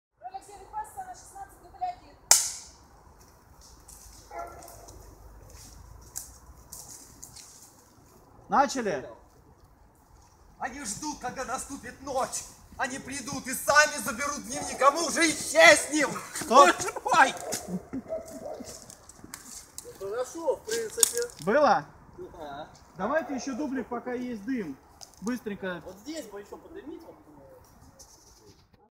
Два билета на тот свет - Бекстейдж - Герои мужественно покидают кладбище
Съемки на старом кладбище. Автор Видео - Михаил Иванов.